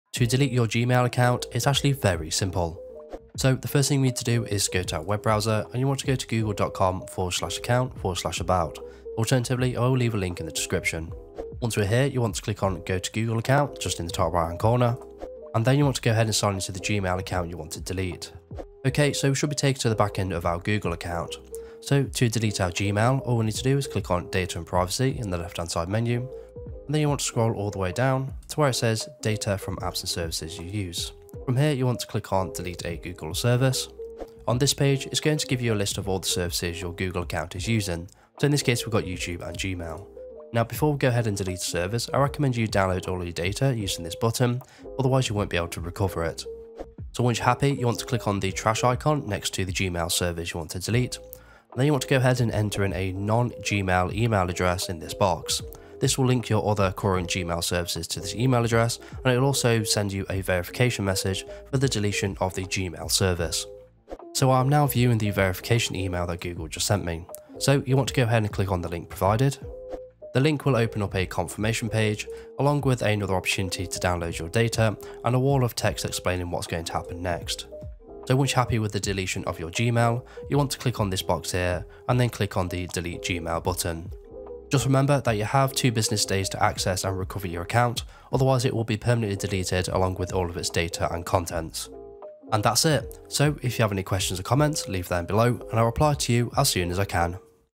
How To: Permanently Delete Gmail Account - (Quick & Easy)
Today, you will successfully learn how to permanently delete your Gmail account. When you delete your gmail account, it's important to note that you'll only have 2 business days to recover your gmail account. If you're like me and struggle to remember passwords, this video is for you.